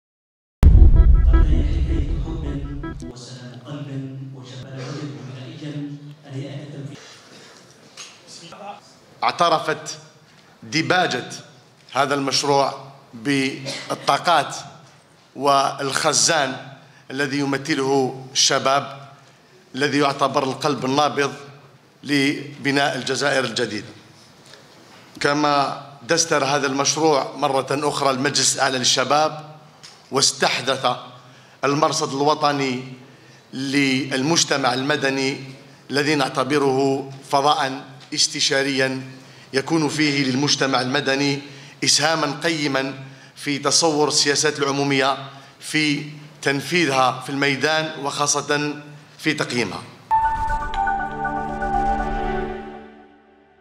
غرداية: خالدي يؤكد أن تعديل الدستور "يؤسس جمهورية جديدة أساسها الشباب"
غرداية (وأج)- أكد وزير الشباب والرياضة ، سيدي عل خالدي، على أن مشروع تعديل الدستور،  الذي سيعرض على الاستفتاء الشعبي، "يؤسس جمهورية جديدة أساسها الشباب."